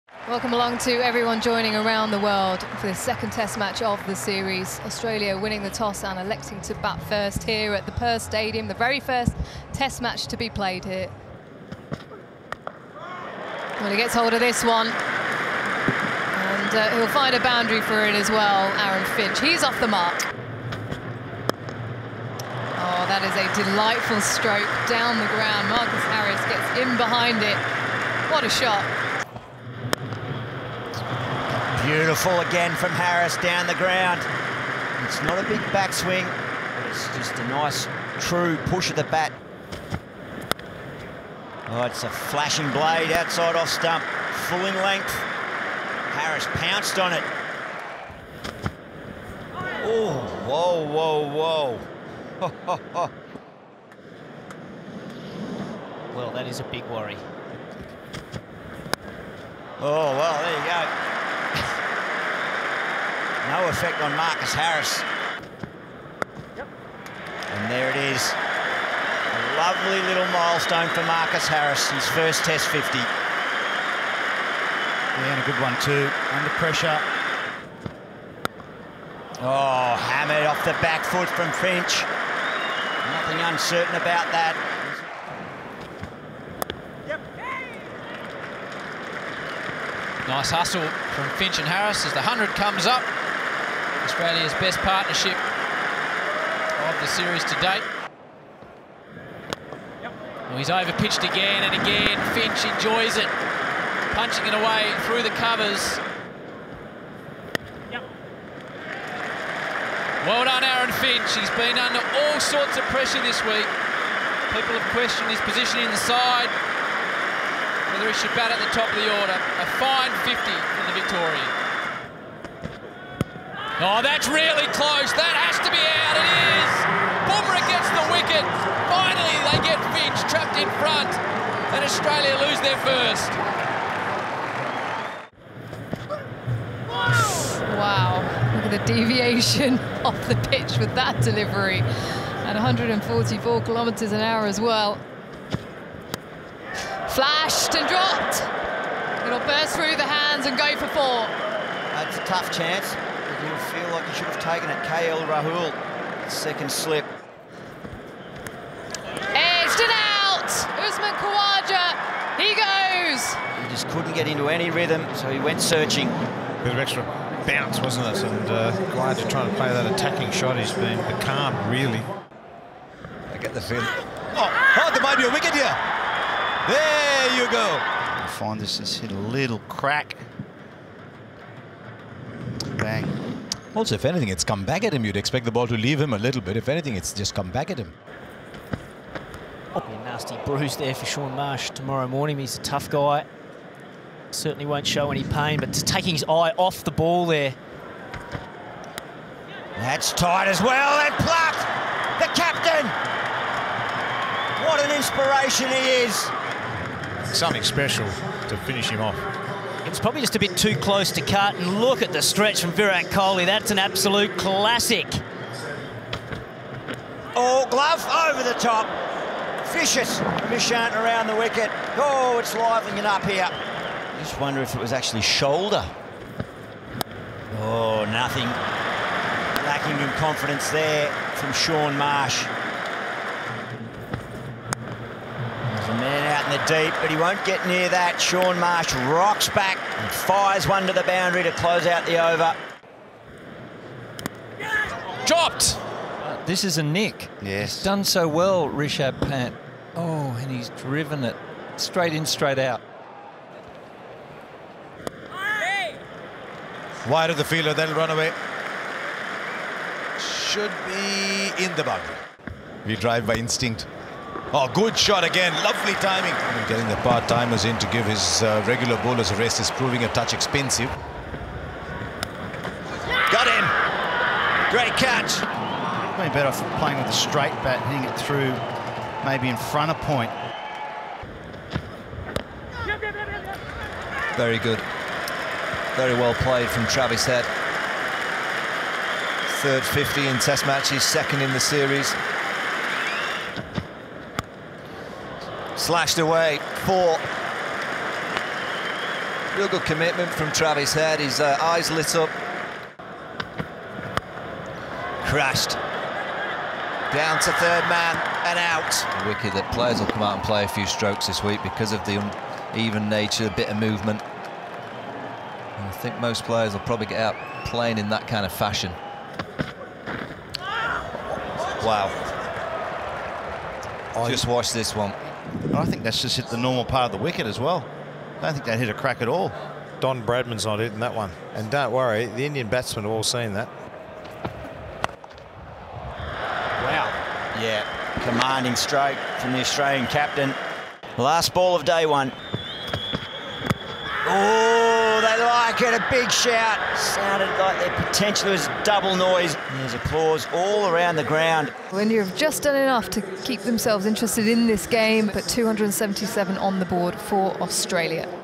Aussies battle hard against valiant India | Second Domain Test
Australia had three batsmen pass fifty but nobody made batting look easy on a difficult pitch as Jasprit Bumrah and the India quicks were on song to make for a captivating day of Test cricket